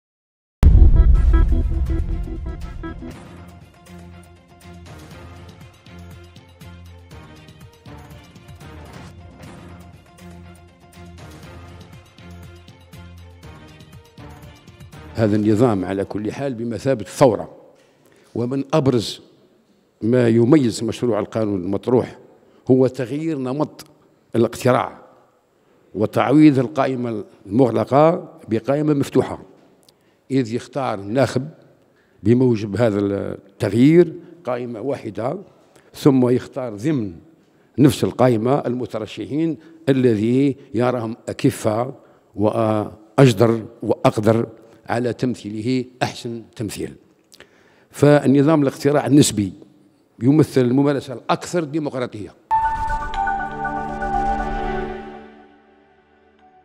فنيش: نظام الاقتراع النسبي يعد ثورة في مشروع قانون الانتخابات
الجزائر (وأج) – أكد رئيس المجلس الدستوري كمال فنيش لدى استضافته في منتدى الإذاعة أن نظام الاقتراع النسبي يعتبر بمثابة ثورة في مشروع قانون الانتخابات.